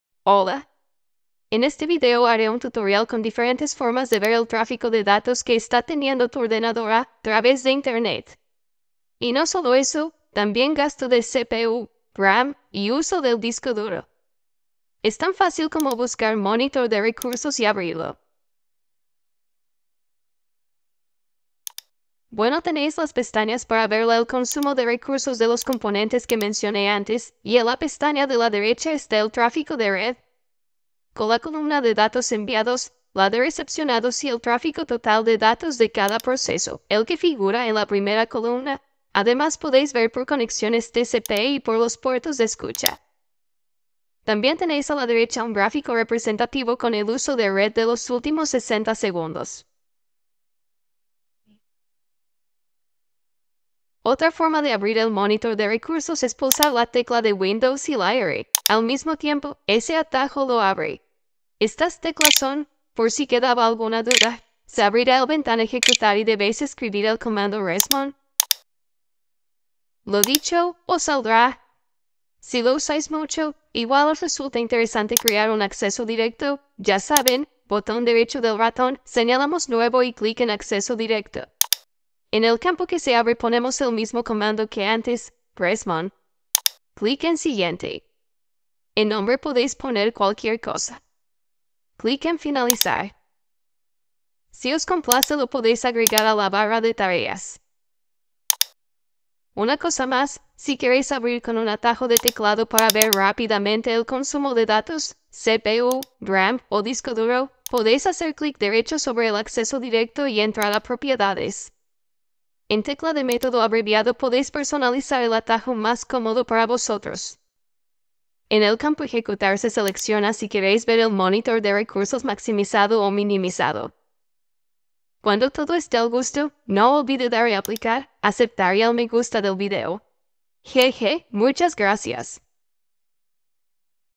Ver en tiempo real qué aplicación o programa consume más datos en segundo plano en Windows PC
En este tutorial, te mostraremos cómo acceder y utilizar el Monitor de Recursos de Windows para supervisar el tráfico de datos, el uso de CPU, RAM y el disco duro de tu ordenador. No necesitas ser un experto en informática, ya que este proceso es tan sencillo como buscar el Monitor de Recursos y abrirlo. ¡Vamos a explorar todas las opciones que te ofrece!

Para abrir el Monitor de Recursos, hay varias formas sencillas de hacerlo. La primera es utilizando el atajo de teclado. Simplemente presiona la tecla de Windows y la tecla "R" al mismo tiempo. Esto abrirá la ventana "Ejecutar". En el campo de texto, escribe "resmon" y presiona "Enter". Verás cómo el Monitor de Recursos se abre al instante.

Una vez que tengas el Monitor de Recursos abierto, encontrarás varias pestañas para supervisar el consumo de recursos de tu ordenador. Para nuestra tarea específica, nos centraremos en la pestaña de la derecha, que muestra el tráfico de red.

En esta pestaña, encontrarás tres columnas clave:

1. Datos Enviados: Muestra la cantidad de datos que tu ordenador está transmitiendo a través de la red.
2. Datos Recibidos: Indica cuántos datos tu ordenador está recibiendo de la red.
3. Tráfico Total de Datos: Esta columna resume el tráfico total de datos de cada proceso.

Además, puedes visualizar el tráfico de red por conexiones TCP y por los puertos de escucha. Esto es especialmente útil si deseas identificar las aplicaciones que están utilizando la red.

En el mismo panel de la pestaña de tráfico de red, encontrarás un gráfico representativo que muestra el uso de la red en los últimos 60 segundos. Este gráfico te permite tener una vista rápida y visual de cómo se está utilizando la red en tu ordenador.

Si utilizas el Monitor de Recursos con frecuencia, es posible que desees crear un acceso directo para acceder rápidamente. Para hacerlo, simplemente haz clic derecho en el escritorio, selecciona "Nuevo" y luego "Acceso directo". En el campo que se abre, escribe el comando "resmon" y sigue las instrucciones. Puedes darle cualquier nombre que desees.

Además, si prefieres utilizar un atajo de teclado personalizado, puedes hacer clic derecho en el acceso directo que acabas de crear, ir a "Propiedades" y personalizar el atajo en la pestaña "Acceso directo". Aquí puedes elegir la combinación de teclas que te resulte más cómoda.

Supervisar el tráfico de datos, el uso de CPU, RAM y el disco duro de tu ordenador es una tarea fundamental para mantenerlo funcionando de manera óptima. Con el Monitor de Recursos de Windows y los atajos que te hemos mostrado, puedes acceder y controlar fácilmente estos aspectos. Si este tutorial te ha resultado útil, ¡no dudes en darle "Me gusta" al vídeo y compartirlo con otros! ¡Gracias por tu atención!